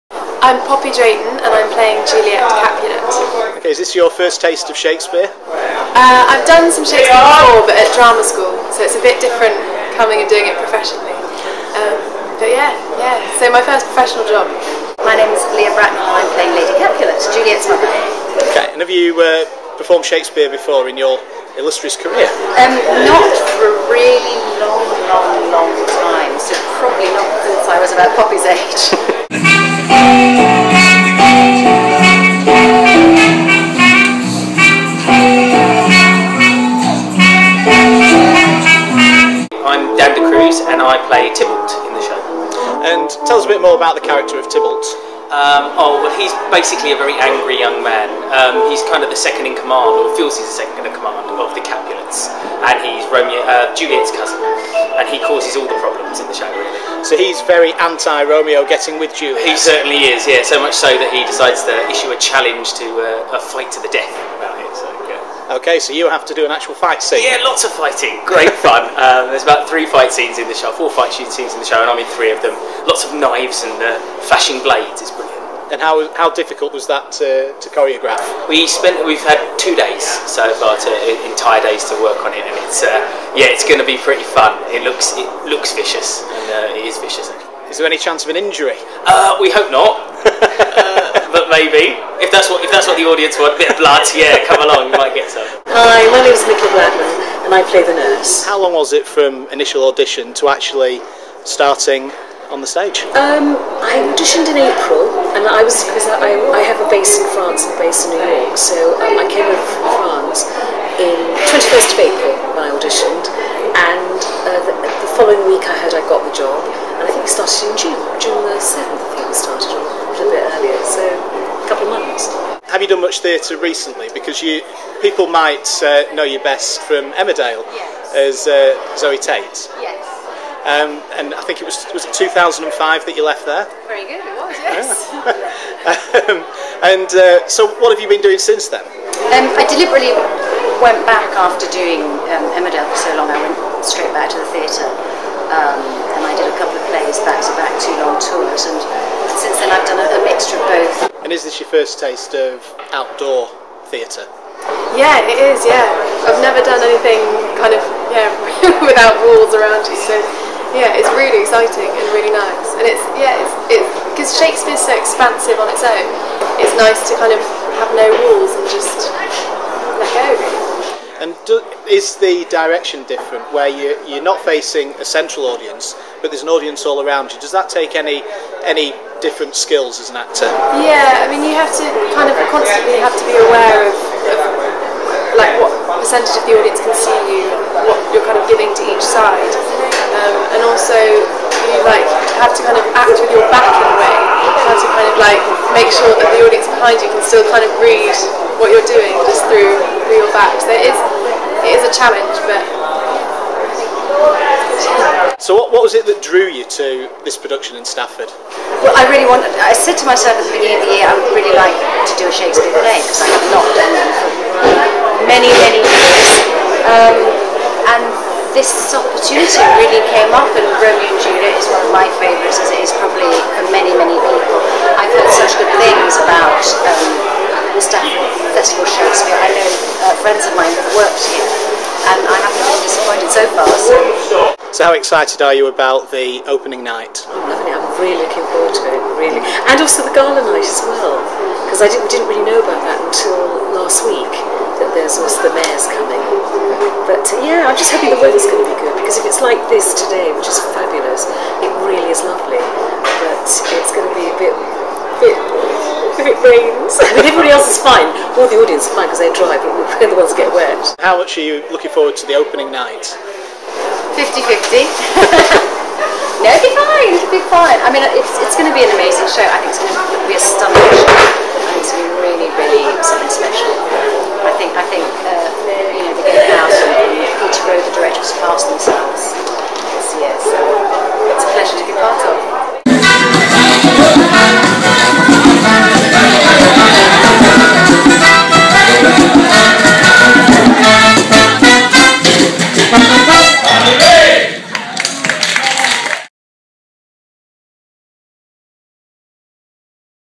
Romeo and Juliet at Stafford Castle 2012
The stars of the Gatehouse Theatre's Romeo and Juliet discuss the production along with some rehearsal footage.